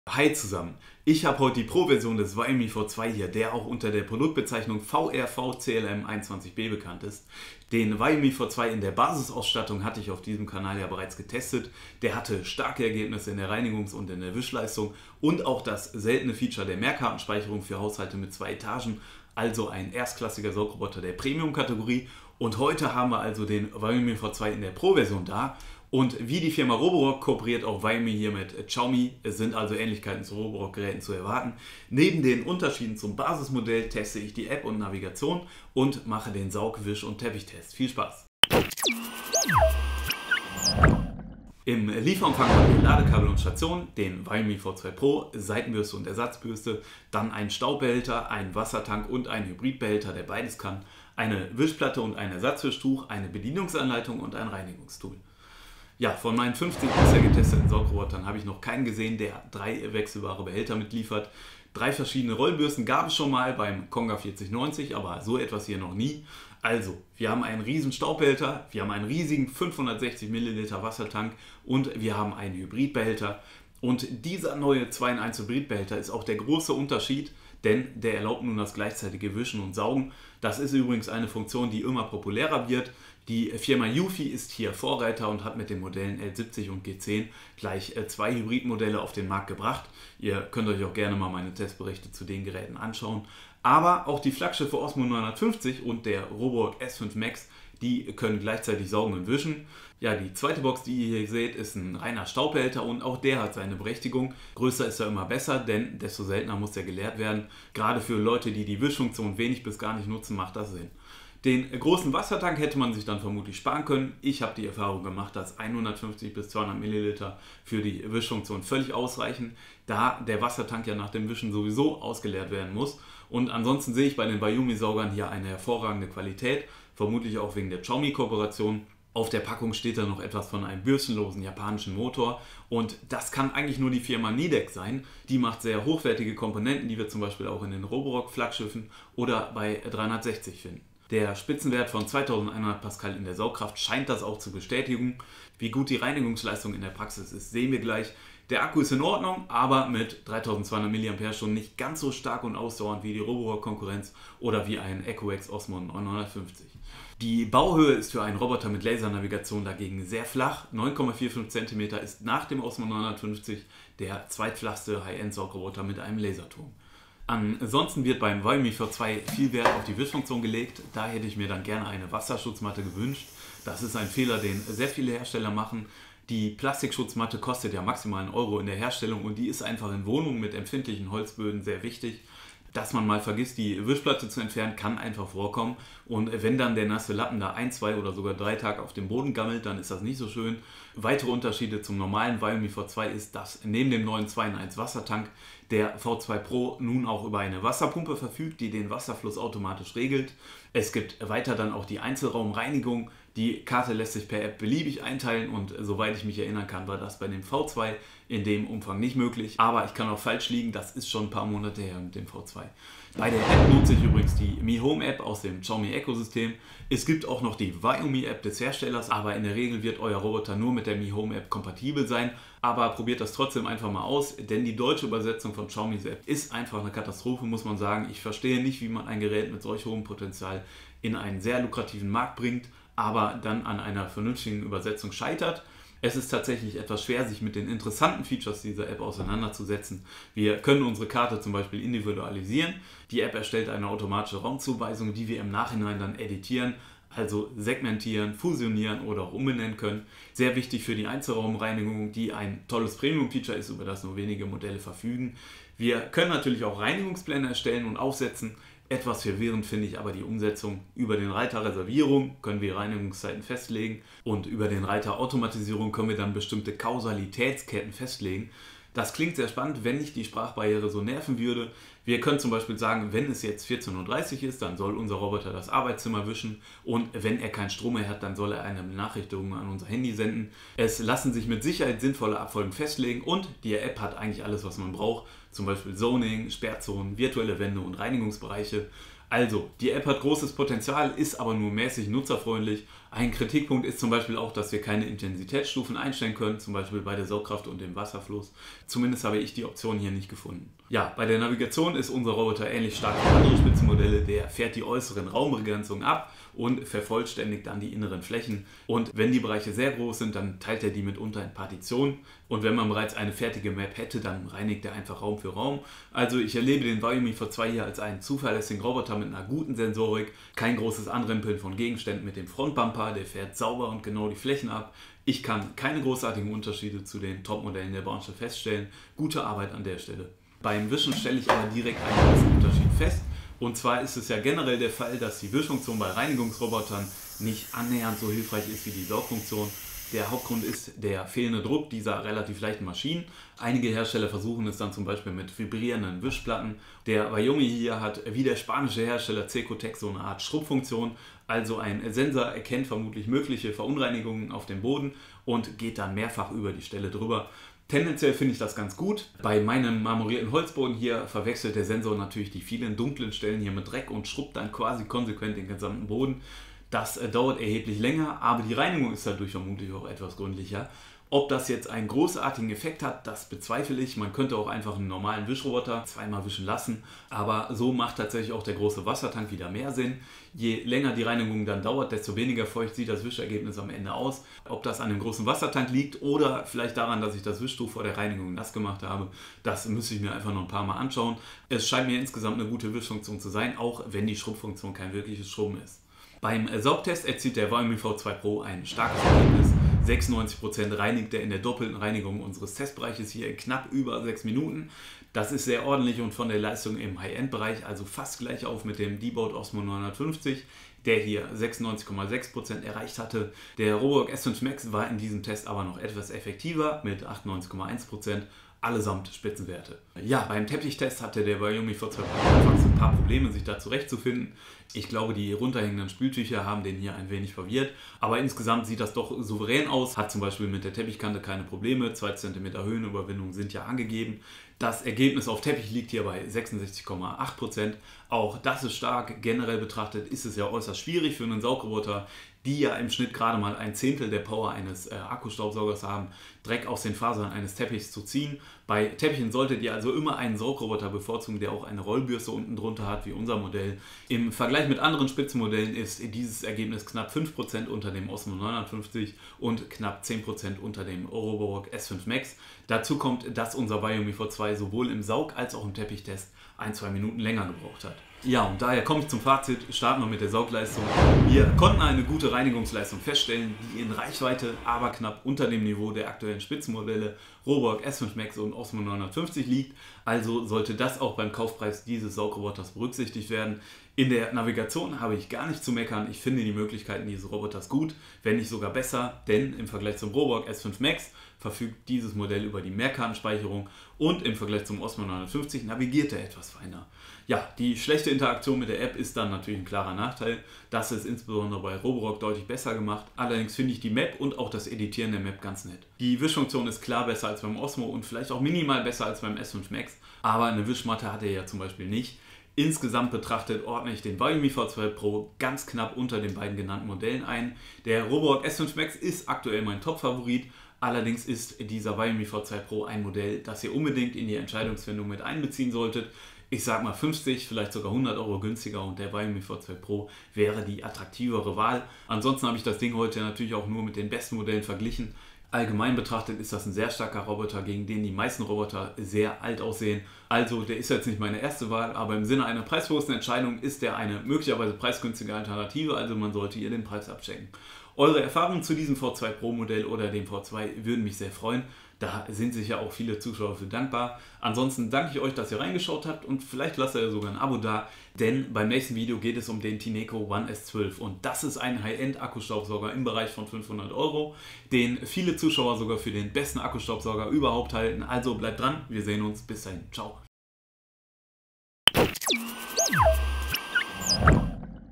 Xiaomi Viomi V2 Pro im Test ► Viomi V2 & Viomi V2 Pro vs Ozmo 950 vs Roborock S5 Max | Testsieger TV
Der Viomi V2 war bereits ein Erfolg. Inwieweit sich der Viomi V2 Pro  (V-RVCLM21B) mit Hybridfunktion und neuen Features gegen die starke Saugroboter Konkurrenz von Roborock & Ecovacs durchsetzen kann seht ihr jetzt anhand von mehreren Tests. Der Test zum Viomi V2 Pro►

Viomi V2 Pro kaufen:

Videoinhalt:
0:00► Einleitung
0:40► Lieferumfang
0:55► Ersteindruck, Produktdaten, Unterschiede V2
3:33► Viomi V2 Pro App  (Mi Home)
5:00► Navigation, Mapping
5:30► Wischfunktion, Wischtest
6:35► Saugtest vs Ozmo 950 & S5 Max
7:00► Teppichtest vs Ozmo 950 & S5 Max
7:47► Fazit

Produktdaten:
► Saugkraft 2100PA
► Bauhöhe 9,45CM
► AKKU 3200mAh
► Ladezeit 180-240Min
► Betriebszeit 60-130Min
► Staubbehälter 550ML
► Wassertank 550ML
► Hybridbehälter 300ML/200ML
► max. Lautstärke 68DB
► Navigation: Laser-Raumvermessung (LDS)
► Hybridfunktion (Saugen&Wischen gleichzeitig)
► Virtuelle Wände , No Go Zonen & Reinigungsbereiche
► Multifloormanagement

Vorteile:
+ Gute Reinigungs- & Wischleistung
+ Preisleistung
+ Flach ( für ein Modell mit Laserturm)
+ Mapping & Zoning
+ Qualität
+ Genaue Navigation / wenig Bumpern
+ Hybrid

Nachteile:
- App / Software
- Keine Wasserschutzmatte
- starke Konkurrenz

Das war es mit dem Testbericht zum Viomi V2 Pro, falls mir oder euch noch das ein oder andere zu diesem Saugroboter auffällt, editiere ich das gerne in die Videobeschreibung rein. Falls ihr Fragen habt, geb ich mein bestes soviele wie möglich in den Kommentaren zu beantworten. Danke! Mark für Testsieger TV